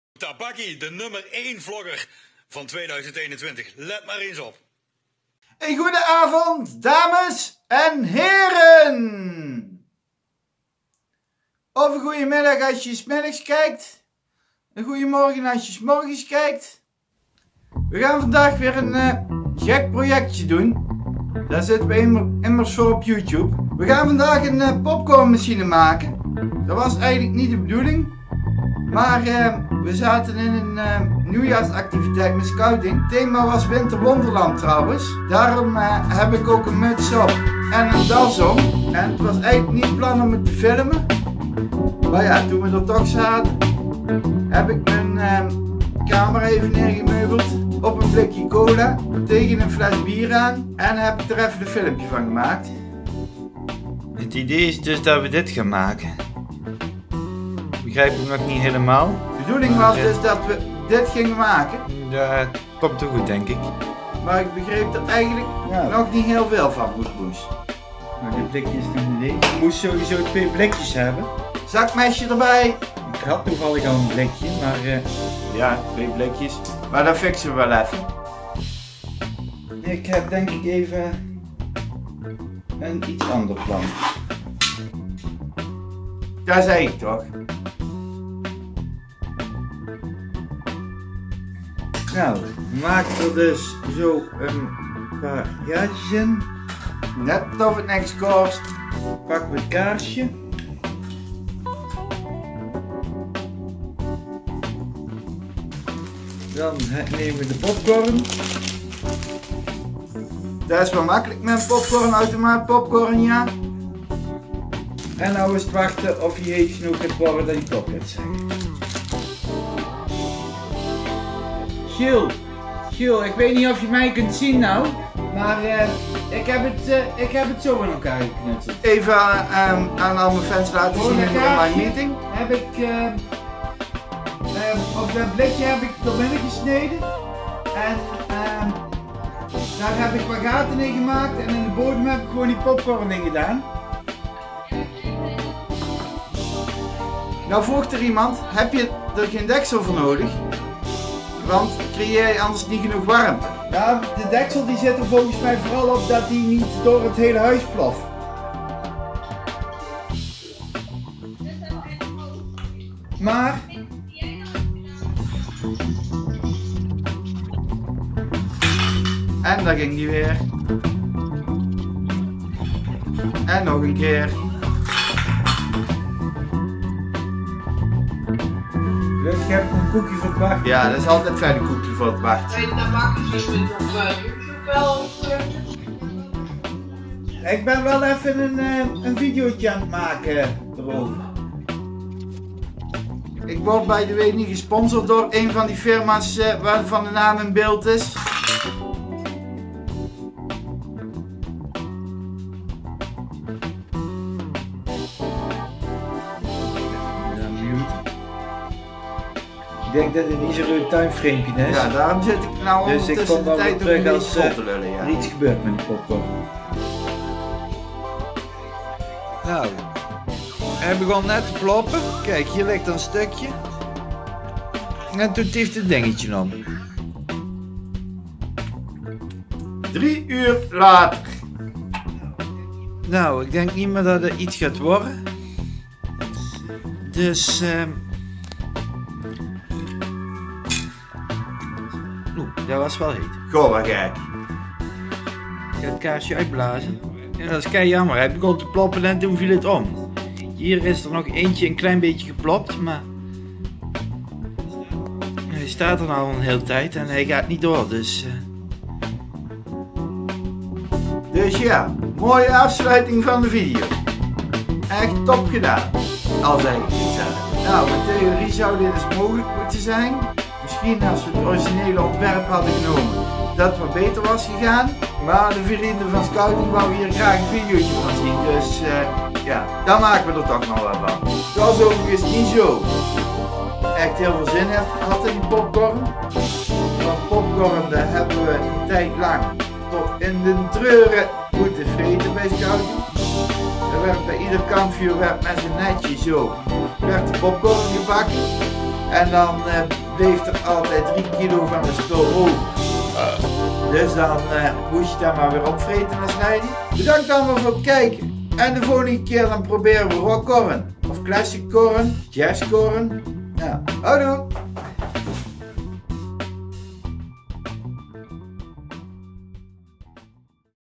Ik maak een Popcorn machine
Tijdens de online nieuwjaar activiteit van scouting kregen we de opdracht op van blikjes een popcorn machine te maken. En omdat er verschillende mensen er al van uitgingen dat ze mijn versie terug zouden zien op YouTube besloot ik maar even snel een geïmproviseerde opstelling te maken om te filmen. Ondertussen reageer ik vanuit de film studio op de gemaakte beelden en geef ik nog wat aanvullende informatie. Ook zo benieuwd of dit wat word of weer in een teleurstelling eindigt? Kijk de video dan nu!